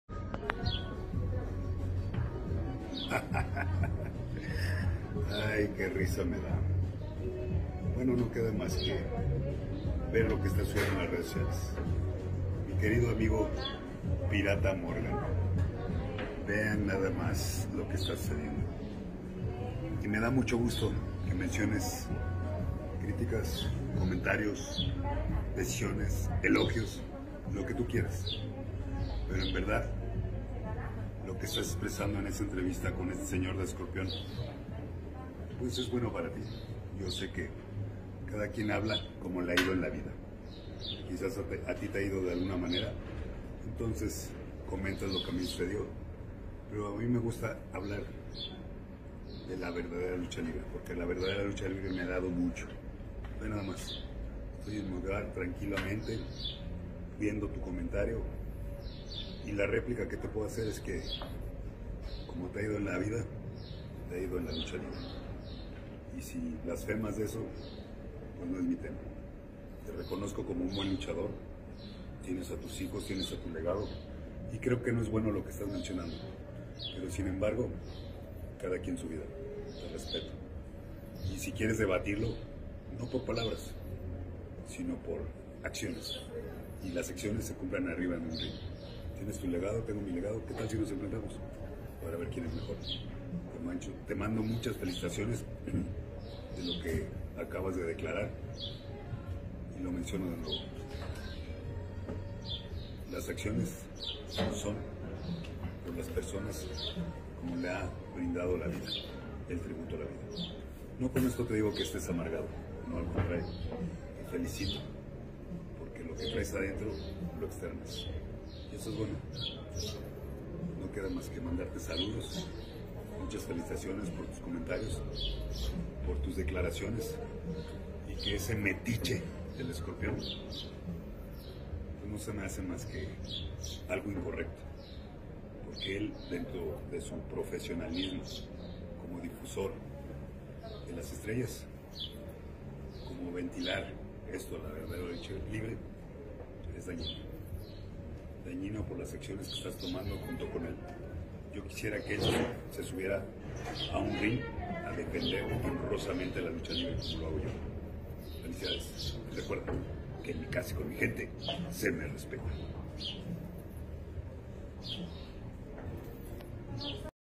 Dr. Wagner Jr. reacciona ante "polémicas" declaraciones de Pirata Morgan con el Escorpion Dorado.
Tras las polémicas declaraciones del autonombrado "El Mejor luchador del Mundo", Pirata Morgan, Dr. Wagner Jr, le responde con este contundente mensaje.